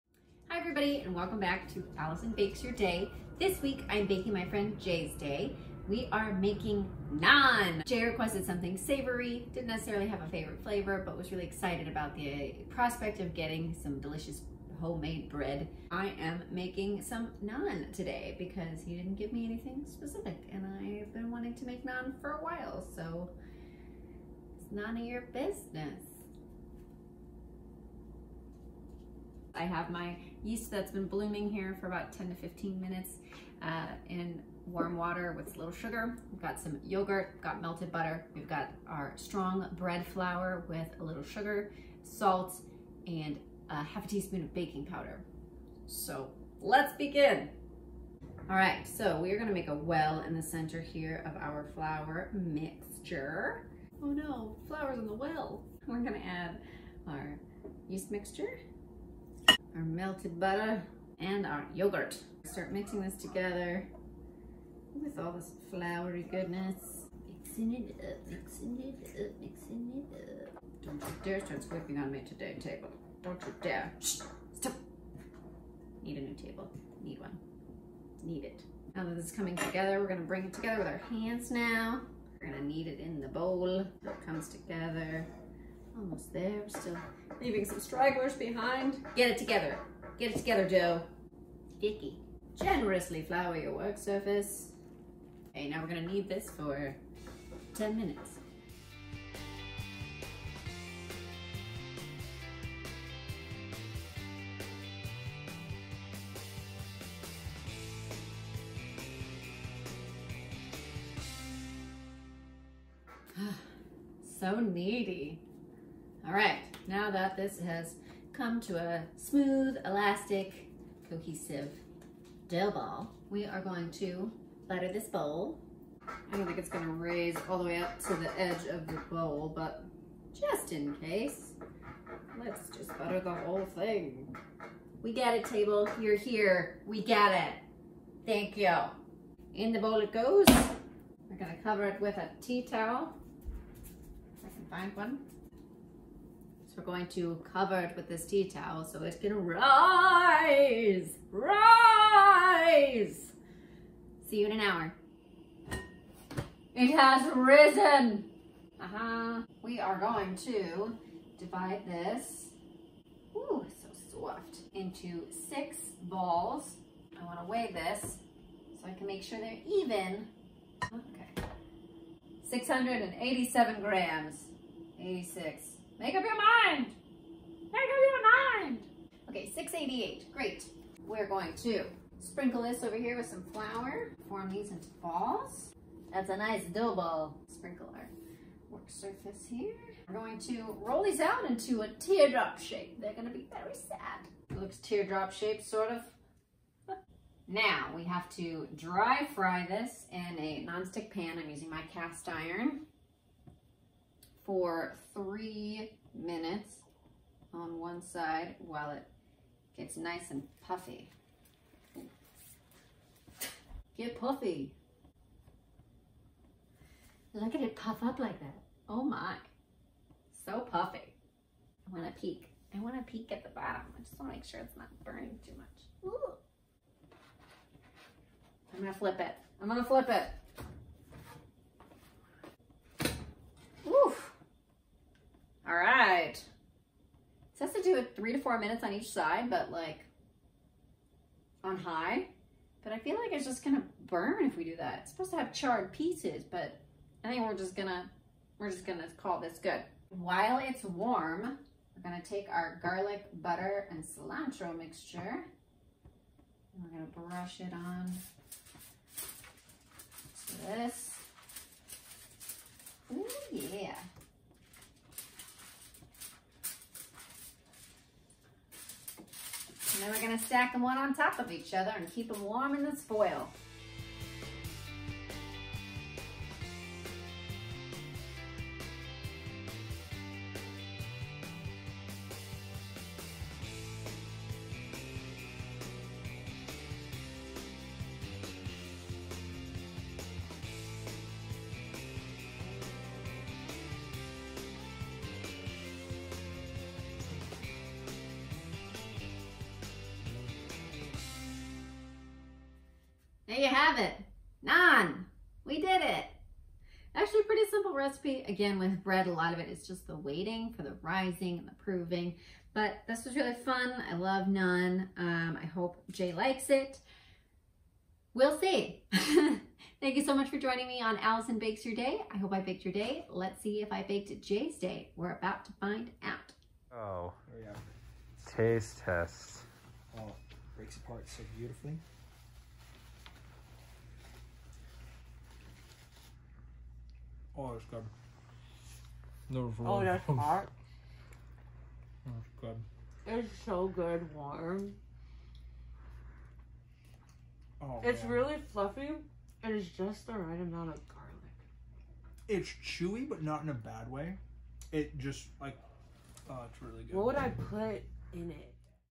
Episode 23: Naan!
Ingredients

1x 7g sachet dried yeast
2 tsp golden caster sugar
300g strong white bread flour, plus extra for dusting
½ tsp baking powder
25g butter or ghee, melted, plus extra 2-3 tbsp for the tray and brushing
150ml natural yogurt
1 tbsp nigella seeds (optional)

Garlic butter
2 tbsp butter melted
4-5 cloves of garlic finely chopped or pressed
A couple sprigs of cilantro chopped
1/4 tsp fine sea salt

Method

1. Put 125ml warm water into a bowl and sprinkle over the yeast and 1 tsp of the sugar. Leave for 10-15 mins or until frothy. In a larger bowl, put the flour, remaining sugar, ½ tsp salt and baking powder. Mix together then make a well in the centre in which to pour the melted butter, yogurt, nigella seeds and yeast mixture. Stir well, then start to bring the mixture together with your hands. If it’s very wet add a spoonful of flour but if it’s dry add a splash more warm water. It should be a very soft dough but not so wet that it won’t come together into a ball.

2. When you’re happy with the consistency, start kneading, first in the bowl, then transfer the mixture onto a well-floured surface and continue to knead for 10 mins or until smooth and elastic but still soft. Butter a large bowl, then shape the dough into a ball and place in the prepared bowl. Cover and leave in a warm place for about 1 hr or until doubled in size.

3. Divide the dough into six balls and put them on a baking tray dusted with flour, then cover the tray with a damp tea towel. Heat a large non-stick frying pan over a high heat. Take one of the balls of dough and roll it out to form a teardrop shape that’s approximately 21cm long and around 13cm at the widest part. When the pan is very hot, carefully lay the naan bread into it. Let it dry fry and puff up for about 3 mins, then turn over and cook on the other side for another 3-4 mins or until cooked through and charred in patches.

4. Heat the oven to its lowest setting and put the cooked naan bread on a baking sheet. Brush with a little garlic butter and cover with foil. Keep warm in the oven and layer up the cooked naans one on top of each other as you make them, brushing each one with melted butter or ghee as you go. Serve warm with curry or dips.

**I didn’t keep them warm in the oven since I wasn’t serving right away. You can reheat them in the oven at 300F/150C for 5 minutes